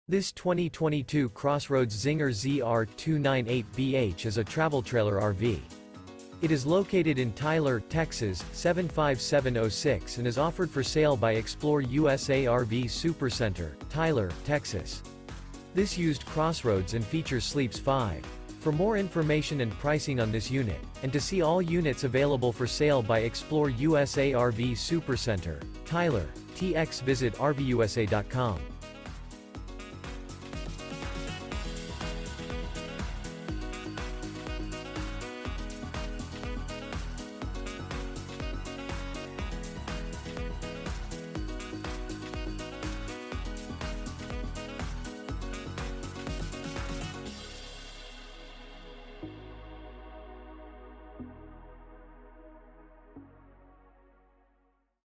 Incredible 2022 CrossRoads Zinger Travel Trailer RV For Sale in Tyler, TX | RVUSA.com
This 2022 CrossRoads Zinger ZR298BH is a Travel Trailer RV. It is located in Tyler, TX 75706 and is offered for sale by ExploreUSA RV Supercenter - TYLER, TX. This Used CrossRoads and features sleeps 5..